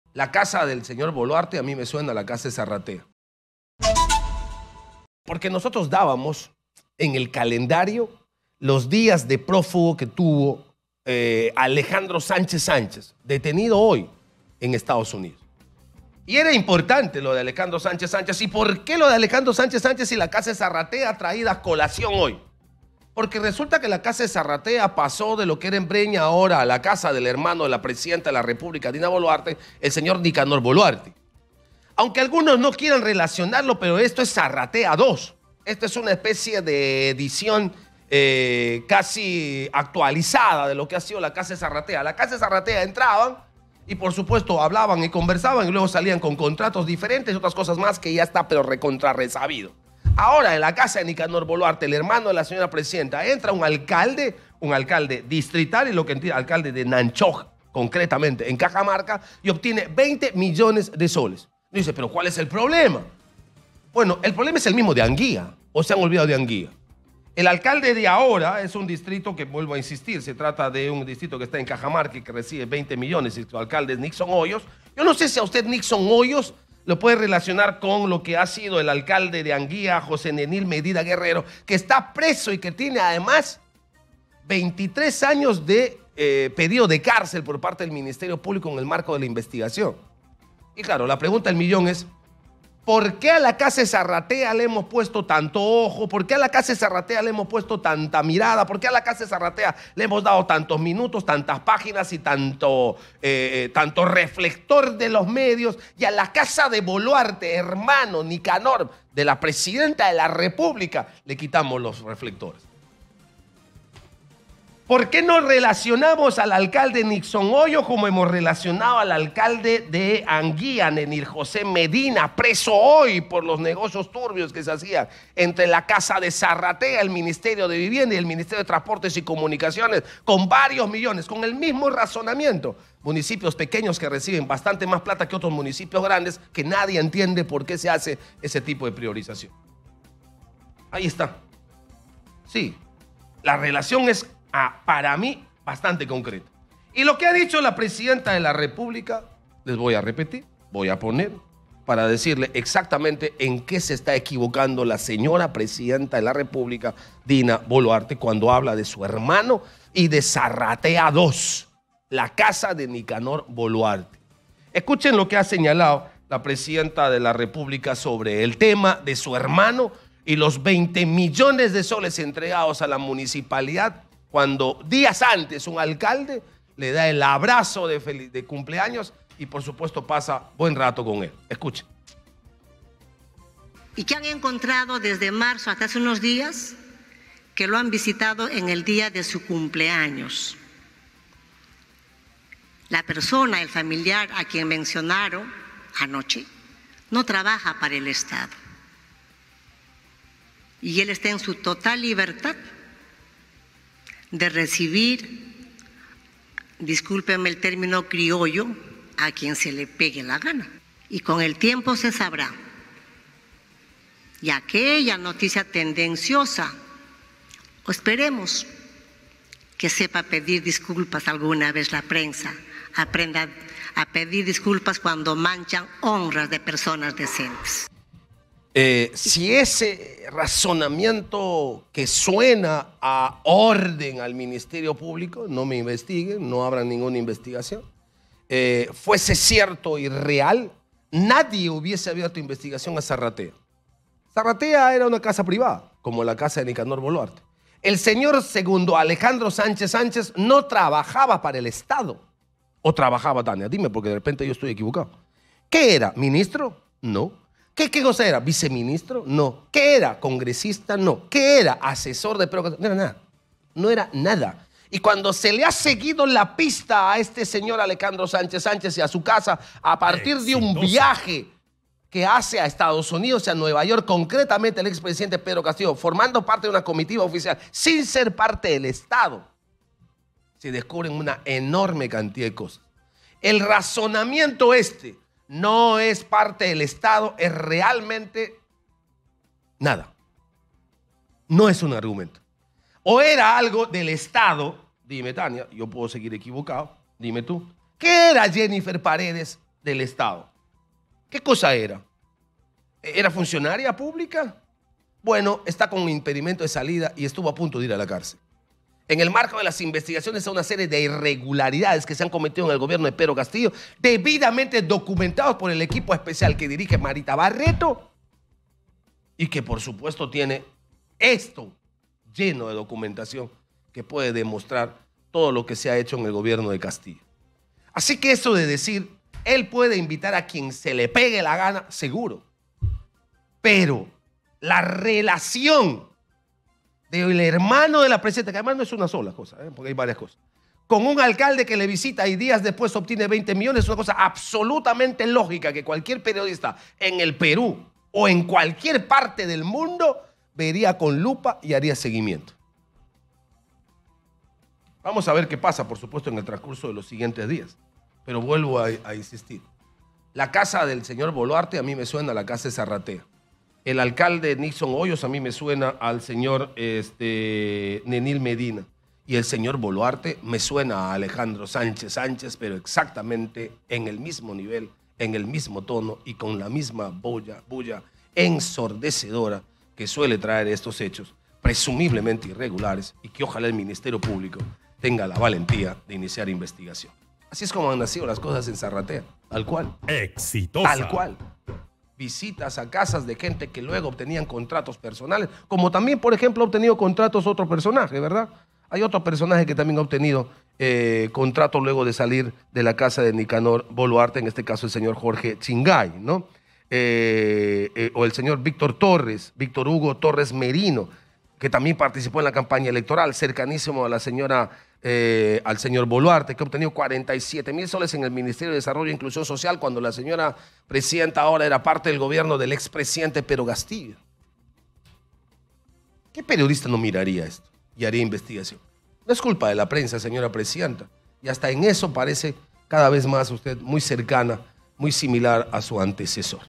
🔴🔵Manuel Rosas: La casa del hermano de la presidenta Dina Boluarte me suena a la casa de Sarratea
El conductor de Exitosa, Manuel Rosas, dijo que el recibimiento del hermano de la presidenta, Nicanor Boluarte, a un alcalde en su casa podría tratarse de un caso similar al de Sarratea.

Exitosa Perú
"Exitosa: La voz que integra al Perú"

Corporación Universal